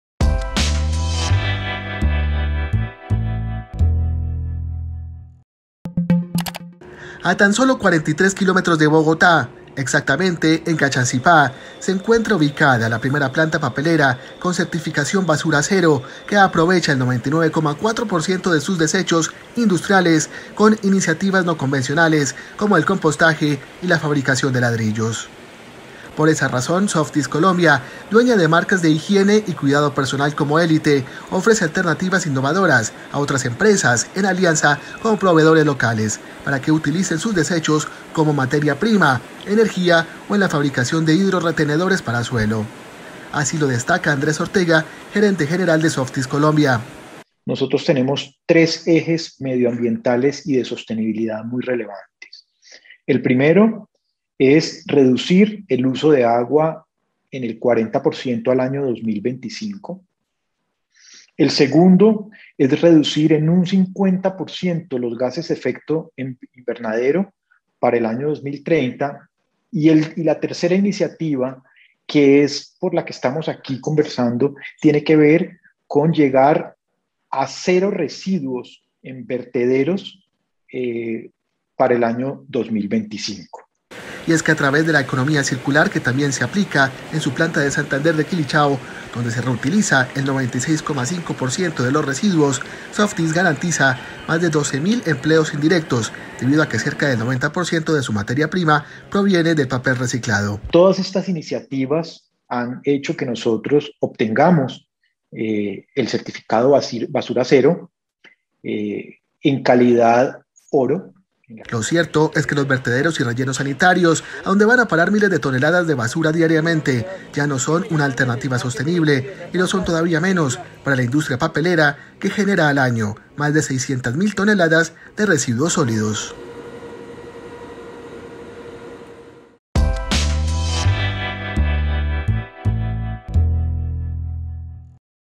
Planta papelera en Colombia aprovecha los desechos industriales como modelo sostenible
En Gachancipá, a 43 kilómetros de Bogotá, Colombia tiene la primera planta papelera con certificación "basura cero" que aprovecha el 99,4 % de sus desechos industriales con iniciativas no convencionales como el compostaje y la fabricación de ladrillos.